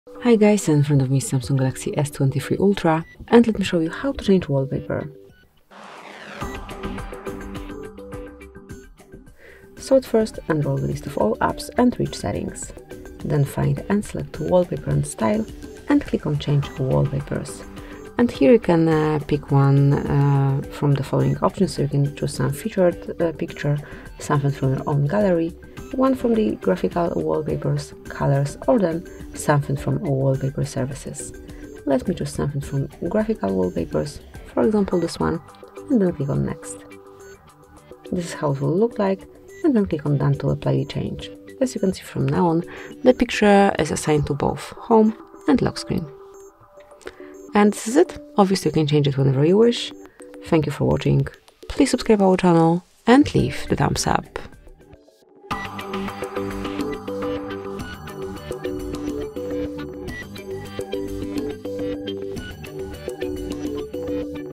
How to Change Wallpaper on SAMSUNG Galaxy S23 Ultra?
Discover the simple process of personalizing your Samsung Galaxy S23 Ultra with custom wallpaper. This tutorial guides you through the steps to change your device's background, from selecting a picture to adjusting it to fit your screen. Upgrade your device's look in just a few clicks!

How to Set Wallpaper on SAMSUNG Galaxy S23 Ultra?
How to Set Up New Wallpaper on SAMSUNG Galaxy S23 Ultra?